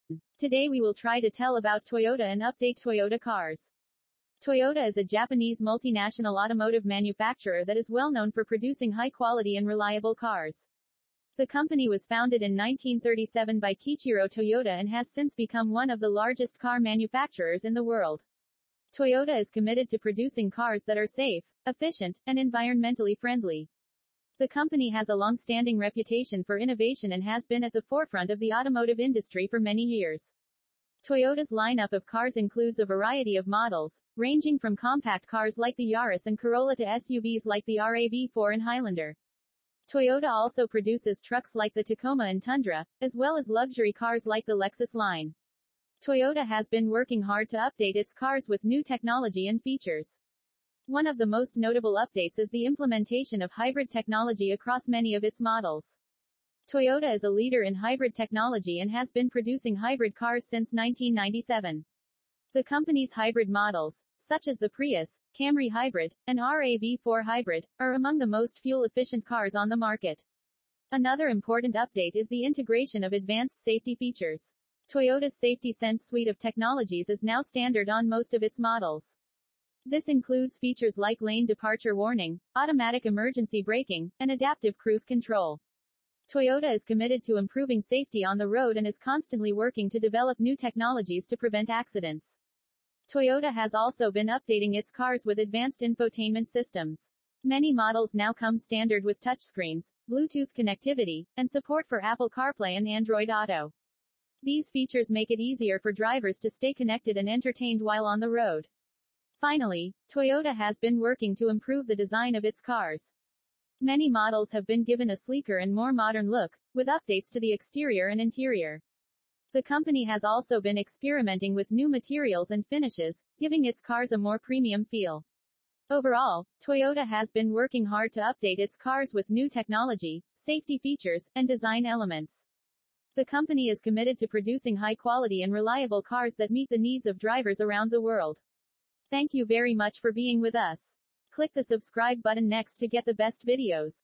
Toyota and update Toyota cars
Assalamualikum.

Welcome everyone.

Today we will try to tell about Toyota and update Toyota cars.

Toyota is a Japanese multinational automotive manufacturer that is well-known for producing high-quality and reliable cars. The company was founded in 1937 by Kiichiro Toyoda and has since become one of the largest car manufacturers in the world. Toyota is committed to producing cars that are safe, efficient, and environmentally friendly. The company has a long-standing reputation for innovation and has been at the forefront of the automotive industry for many years.

Toyota's lineup of cars includes a variety of models, ranging from compact cars like the Yaris and Corolla to SUVs like the RAV4 and Highlander. Toyota also produces trucks like the Tacoma and Tundra, as well as luxury cars like the Lexus line.

Toyota has been working hard to update its cars with new technology and features. One of the most notable updates is the implementation of hybrid technology across many of its models. Toyota is a leader in hybrid technology and has been producing hybrid cars since 1997. The company's hybrid models, such as the Prius, Camry Hybrid, and RAV4 Hybrid, are among the most fuel-efficient cars on the market.

Another important update is the integration of advanced safety features. Toyota's Safety Sense suite of technologies is now standard on most of its models. This includes features like lane departure warning, automatic emergency braking, and adaptive cruise control. Toyota is committed to improving safety on the road and is constantly working to develop new technologies to prevent accidents.

Toyota has also been updating its cars with advanced infotainment systems. Many models now come standard with touchscreens, Bluetooth connectivity, and support for Apple CarPlay and Android Auto.

Finally, Toyota has been working to improve the design of its cars. Many models have been given a sleeker and more modern look, with updates to the exterior and interior. The company has also been experimenting with new materials and finishes, giving its cars a more premium feel.

Overall, Toyota has been working hard to update its cars with new technology, safety features, and design elements. The company is committed to producing high-quality and reliable cars that meet the needs of drivers around the world.

Thank you very much for being with us.

Everyone will be fine.

Assalamualikum.